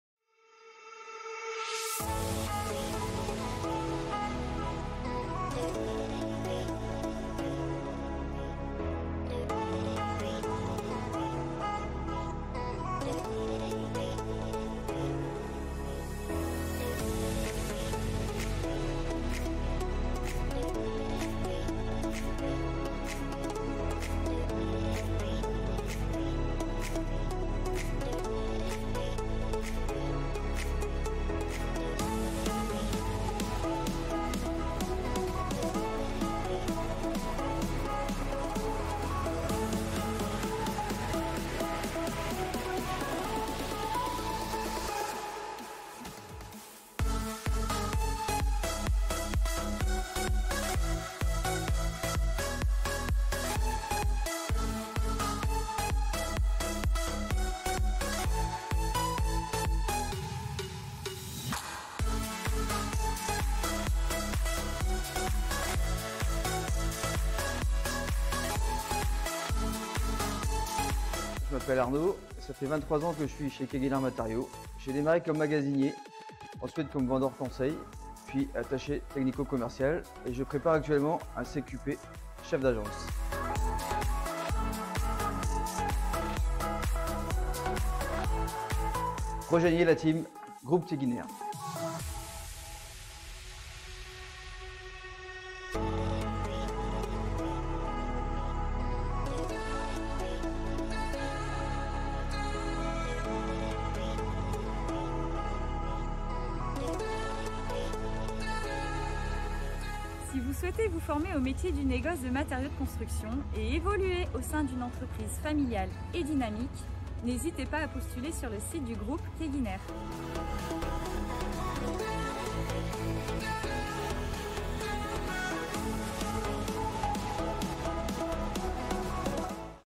Zoom sur le Centre de Formation du Groupe Quéguiner 🔎
La campagne de recrutement pour la formation Vendeur.euse-Conseil a démarré !